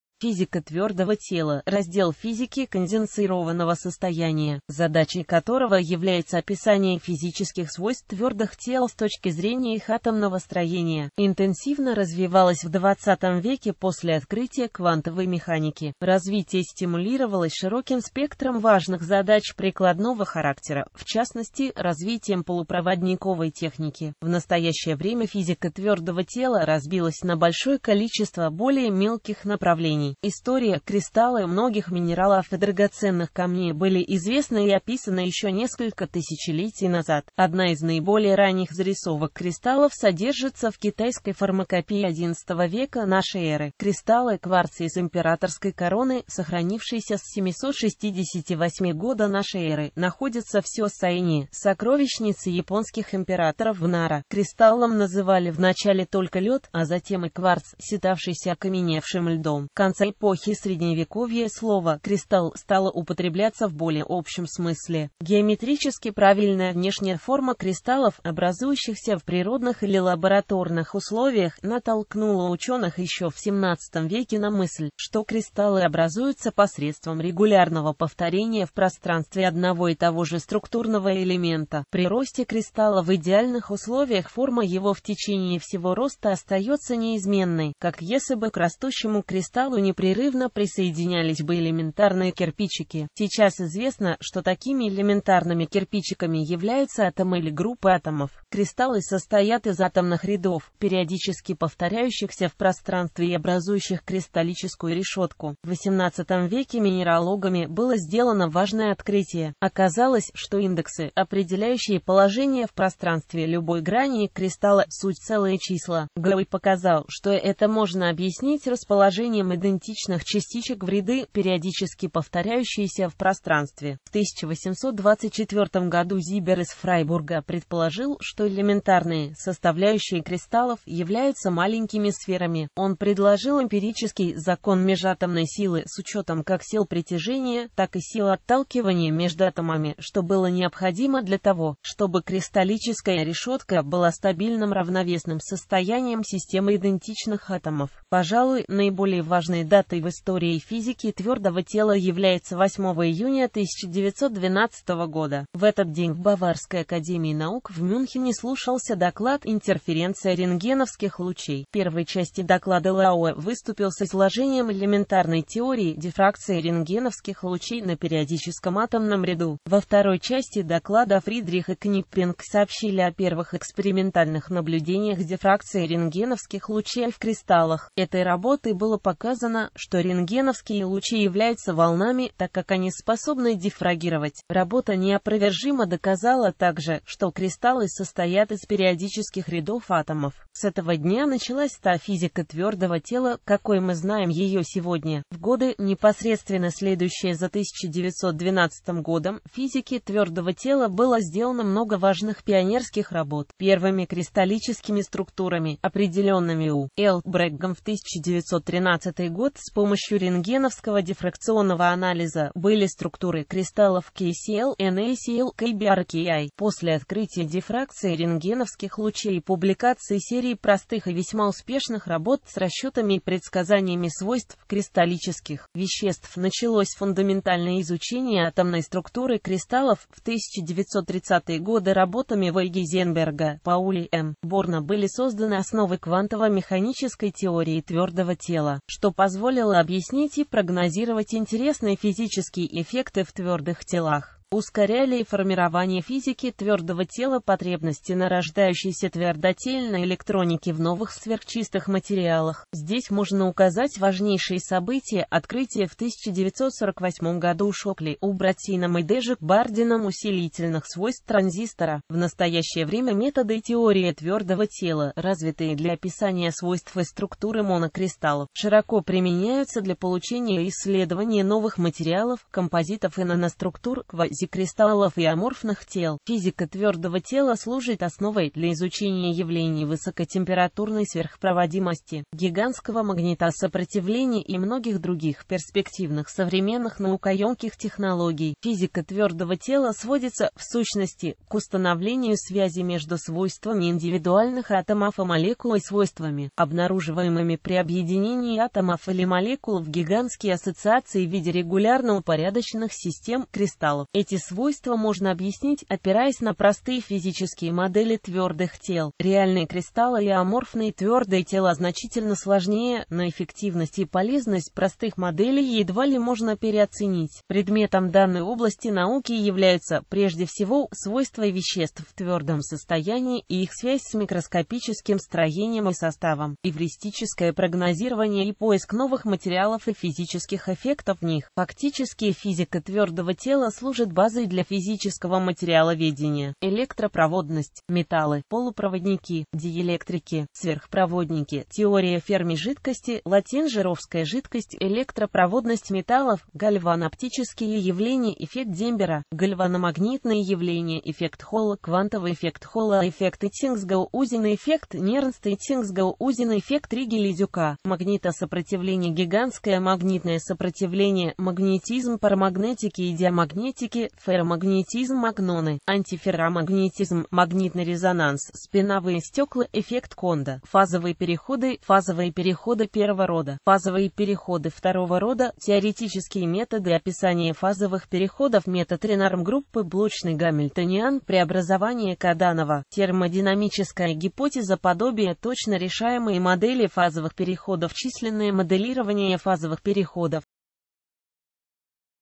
Физика твёрдого тела
Фи́зика твёрдого те́ла — раздел физики конденсированного состояния, задачей которого является описание физических свойств твёрдых тел с точки зрения их атомного строения.Интенсивно развивалась в XX веке после открытия квантовой механики.Развитие стимулировалось широким спектром важных задач прикладного характера, в частности, развитием полупроводниковой техники.В настоящее время физика твёрдого тела разбилась на большое количество более мелких направлений.

Профиль автора: Unknown USGS employee